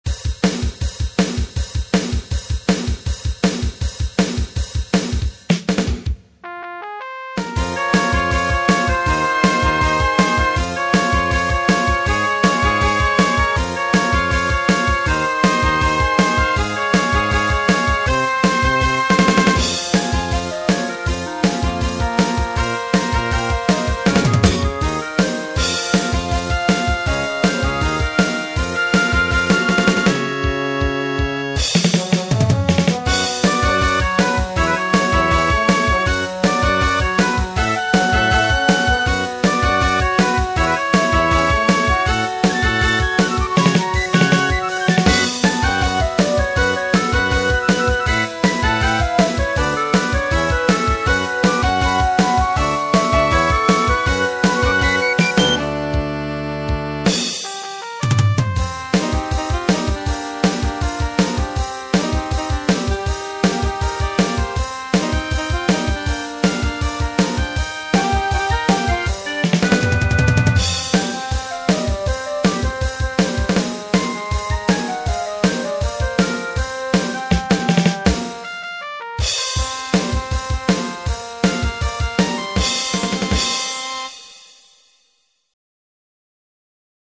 哈哇伊~~嗚喔!!!
花了四天~~~掐頭去尾移除腹內solo. 大概完成適合我們編制的編曲!! 不過還很多聲部沒填滿就是了